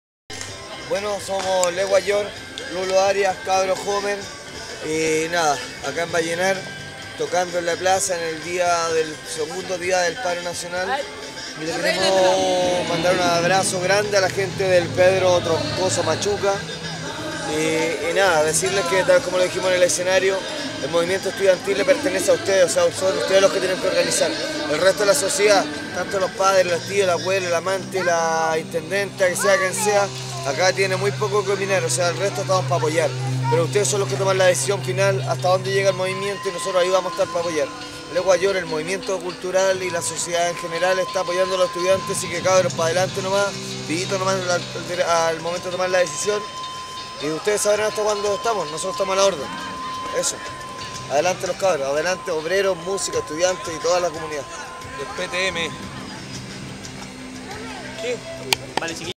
S4030465.AVI
LEGUIAYORK MANDA UN SALUDO A LOS ALUMNOS DEL PEDRO TRONCOSO MACHUCA EN PARO,, EN VALLENAR